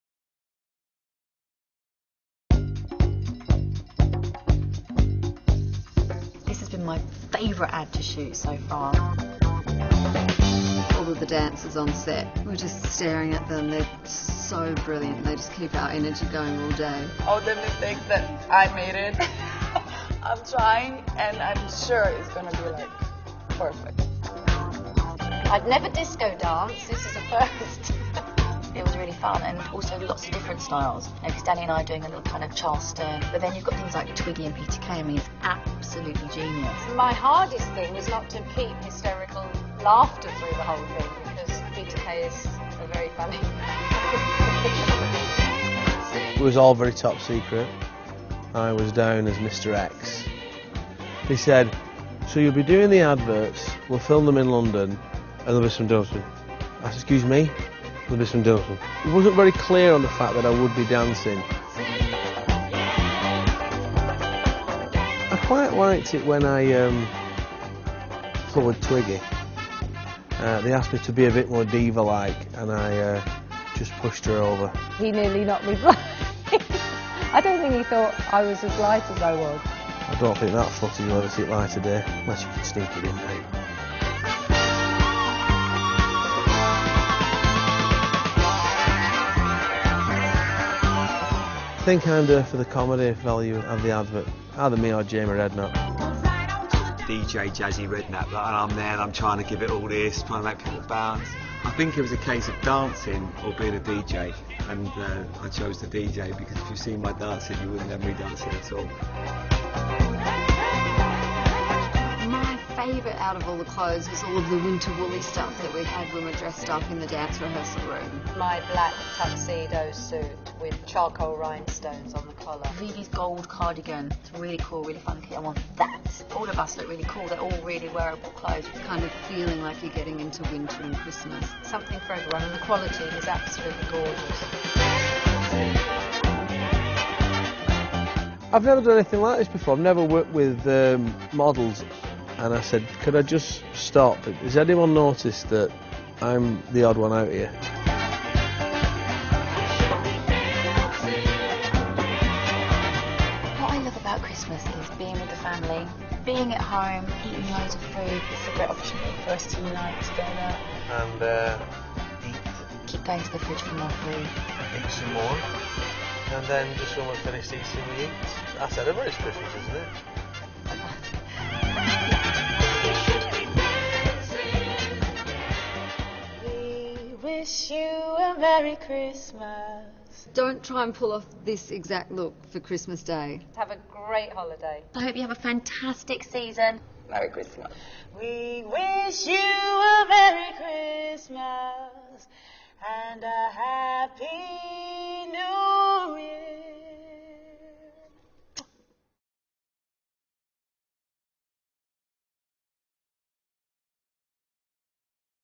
Making of Marks & Spencer Christmas 2010 with Ana Beatriz Barros
Check out Twiggy, Lisa Snowdon, Ana Beatriz Barros, VV Brown, Dannii MInogue, Jamie Redknapp and Peter Kay in the making of for the 2010 Marks & Spencer Christmas commercial!